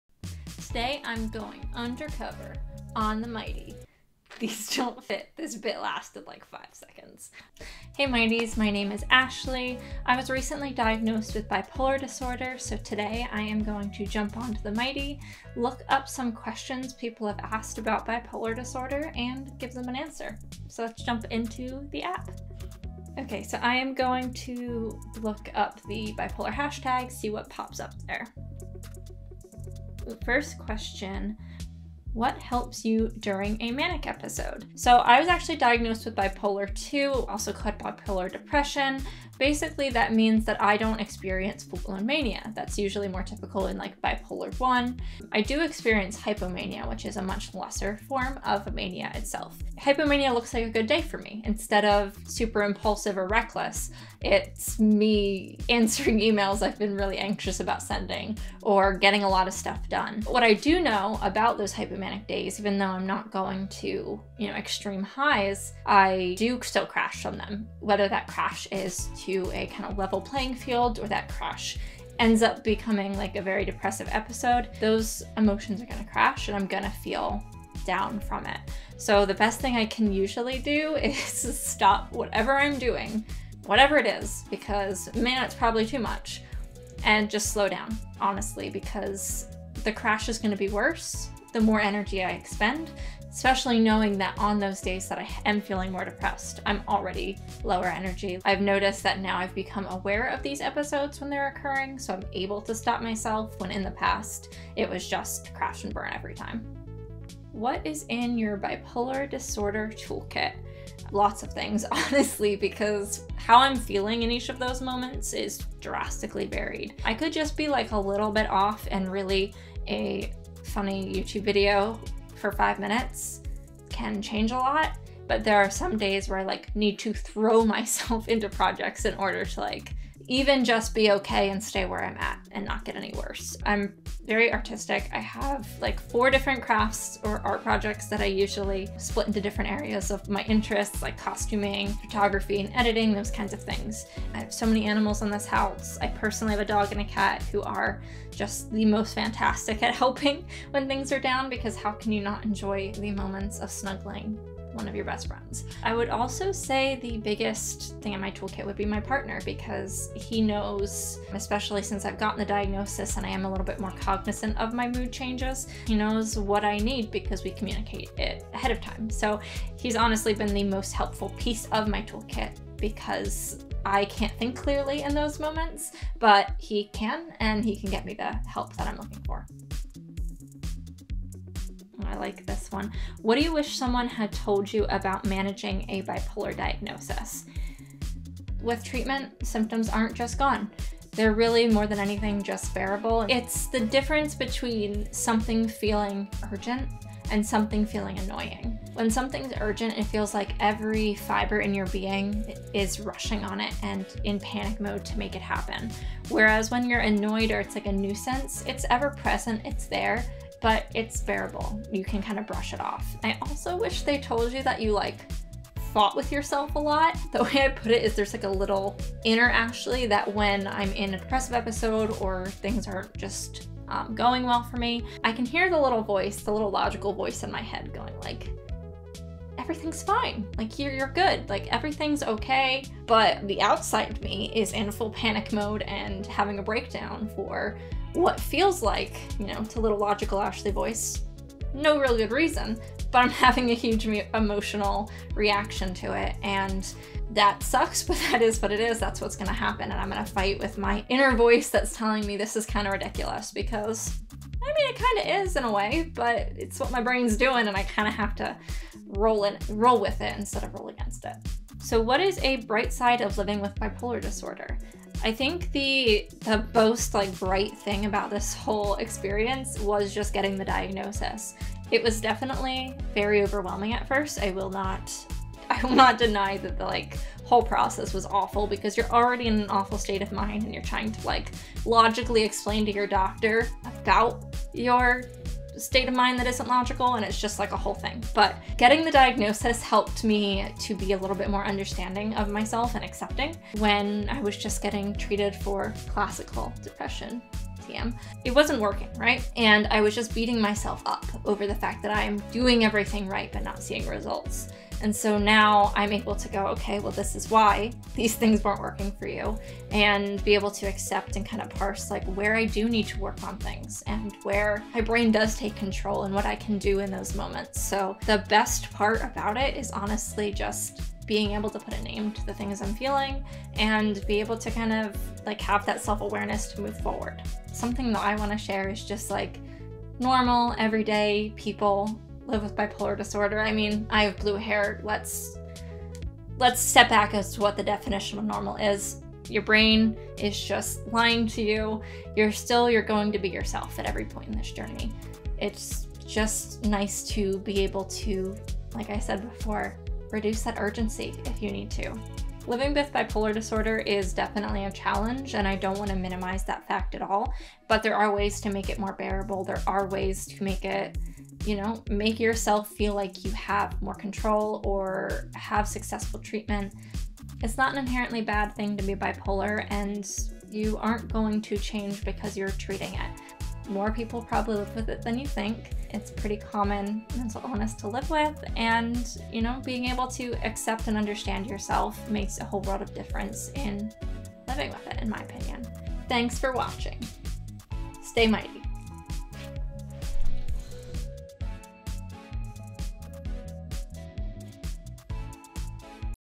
Going Undercover on Bipolar Disorder
After her recent diagnosis, Mighty staffer Ashley Kristoff is going undercover on The Mighty to find out what burning questions you have about bipolar disorder from what helps her during a manic episode to the cool items located in her bipolar disorder “toolkit!" Watch and discover more about bipolar as Ashley goes undercover.

TheMighty.com
Facebook.com/TheMightySite